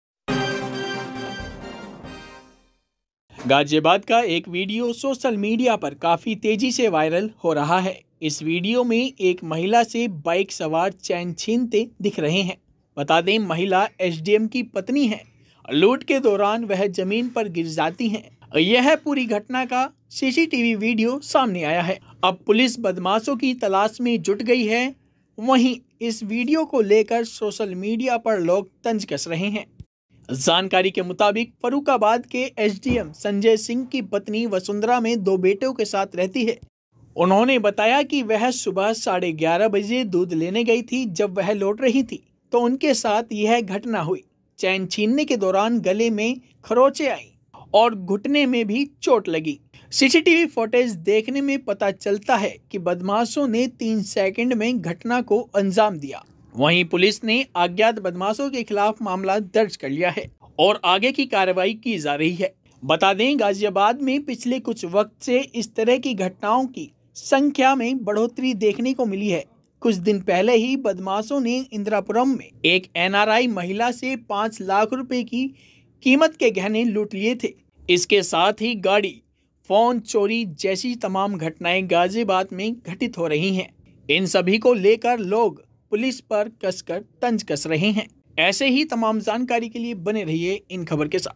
Chain Snatching With SDM Wife: दिनदहाड़े SDM की पत्नी से लूट, चेन लूटकर भागे लुटेरे | Viral Video
गाजियाबाद का एक वीडियो सोशल मीडिया वायरल हो रहा है। इस वीडियो में एक महिला से बाइक सवार चैन छीनते दिखाई दे रहे हैं। महिला SDM की पत्नी है, लूट के दौरान वह जमीन पर गिर गईं। यह घटना सीसीटीवी कैमरे में कैद हो गई, अब पुलिस बदमाशों की तलाश में जुटी हुई है। अब इस वीडियो को लेकर सोशल मीडिया पर लोग तंज कस रहे हैं।